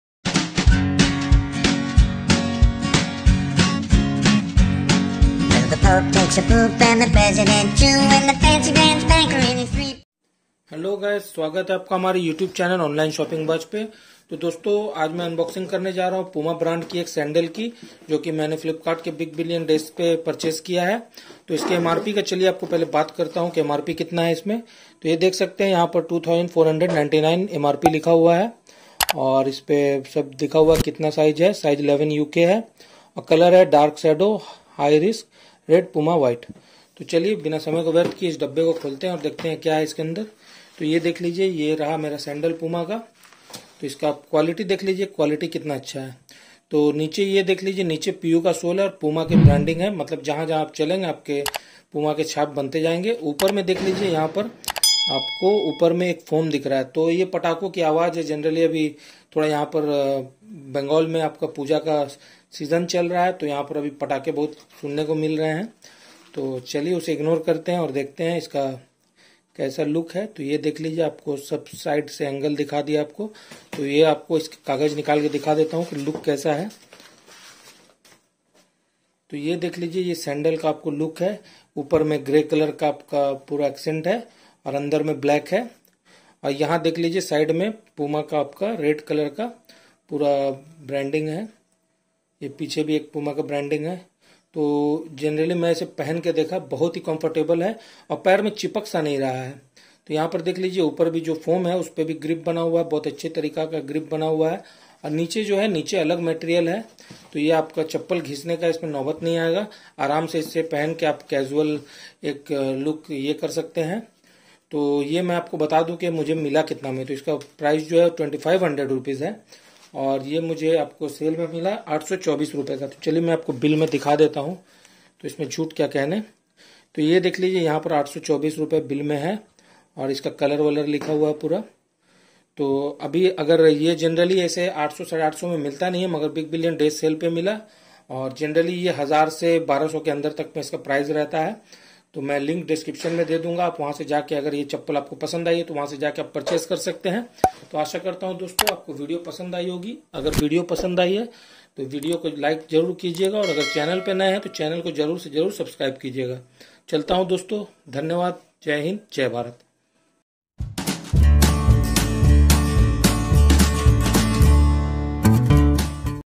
Puma Sandal Unboxing and Review.
Hello Guys,
In this video i did unboxing and review of Puma High Risk Grey Sandal

Available in 2 color variants

Best Price -850 ₹ INR
MRP- 2499 ₹

Wait for Big Diwali Sale

Purchase Link-
Black - Take a look at this PUMA Men Black Sports Sandals on Flipkart

Grey- Take a look at this PUMA Men Grey Sports Sandals on Flipkart